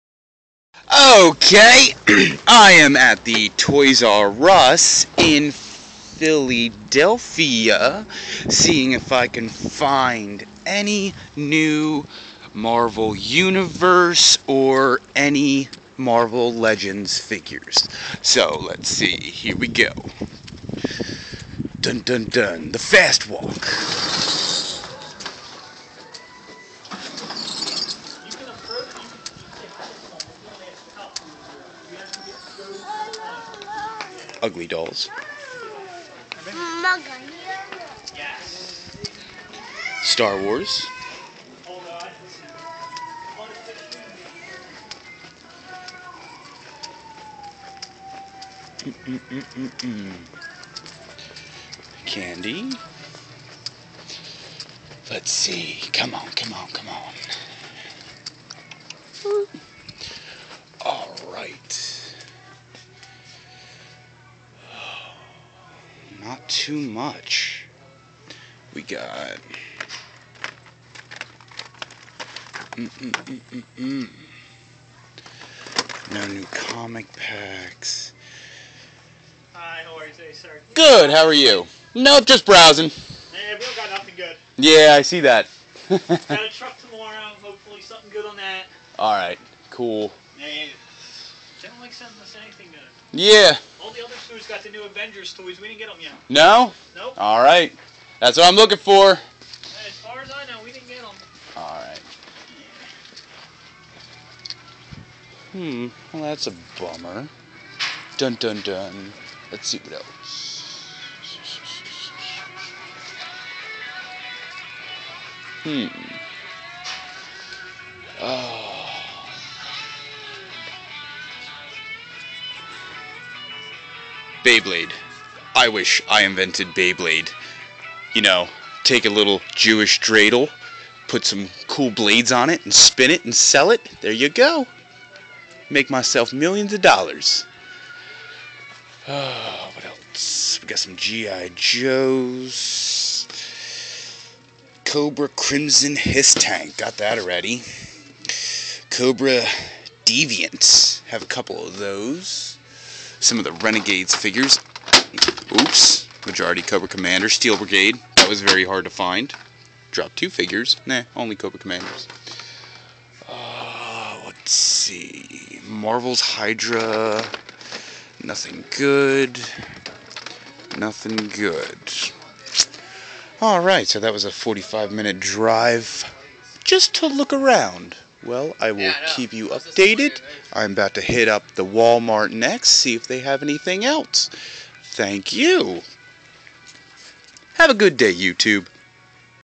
The hunt for new marvel figures.
Went to the toys r us in philly to try and find the new marvel universe, marvel legends, or avenger figures.